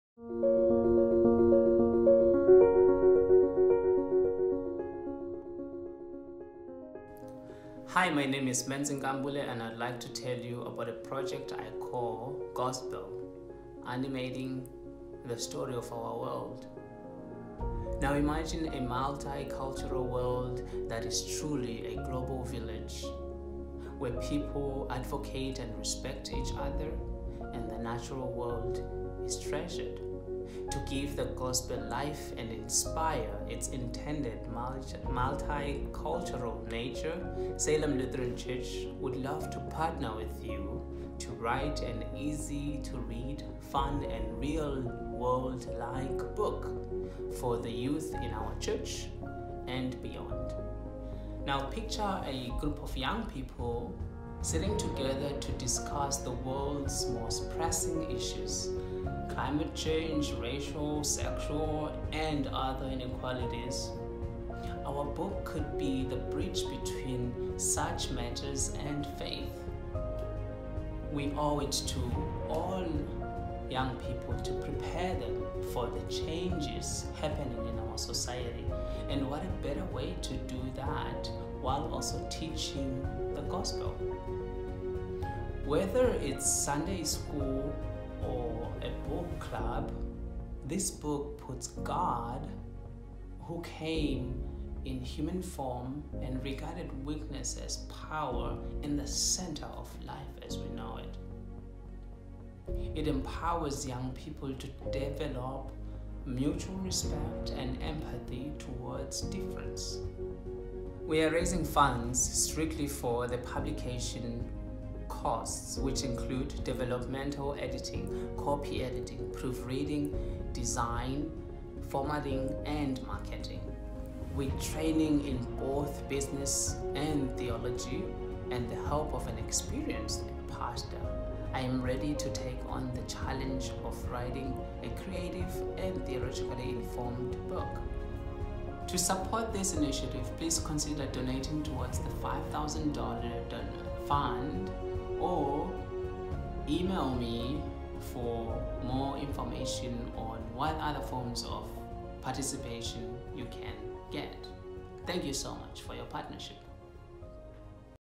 Gospel Project video
This is a pitch for a campaign designed for my Funding Forward class.